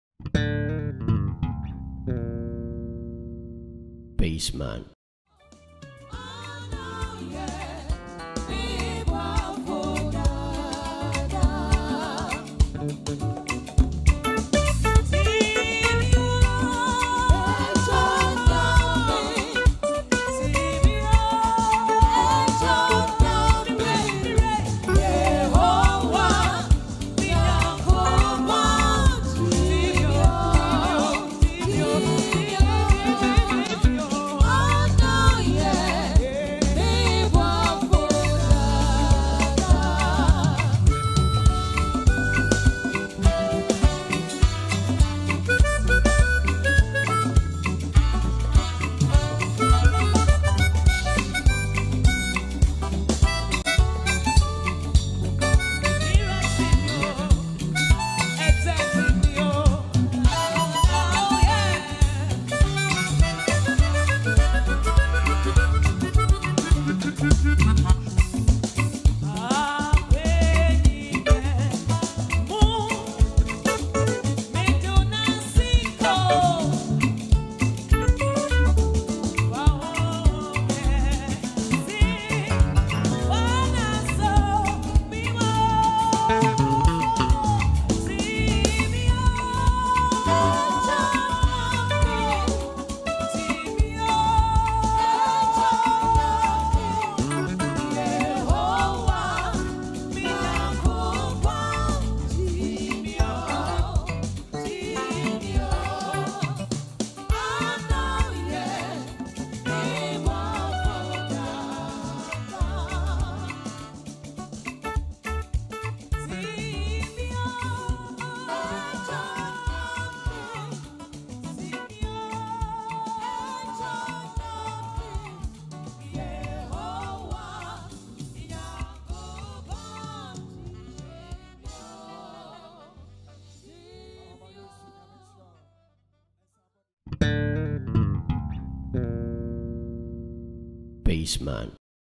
Ike The BassMan🎸 & James Mireku held the groove tight as Lady Rev Lucy performs @Agya Yaw Funeral
REST IN PEACE THE LEGENDARY AGYA YAW